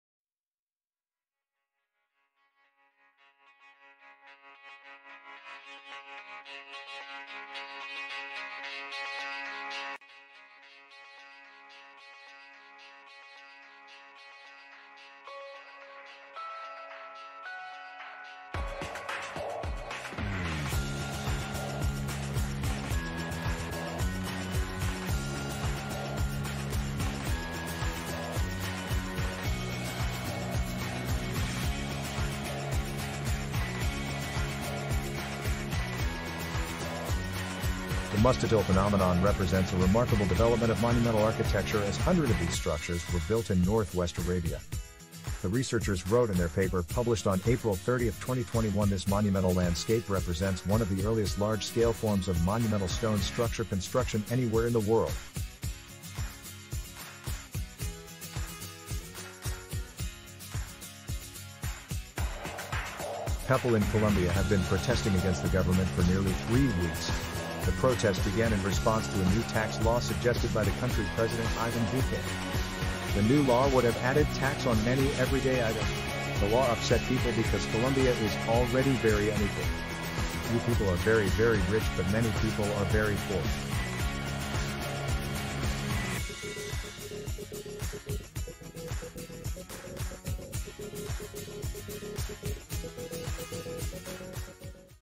WHITE PAINT HELP REDUCE GLOBAL WARMING | GLOBAL STORIES 20052021 | VIKKIDZ
White Paint help reduce Global Warming
World Oldest Monuments Found in Saudi Arabia
Columbian Protest Continues Despite Violence.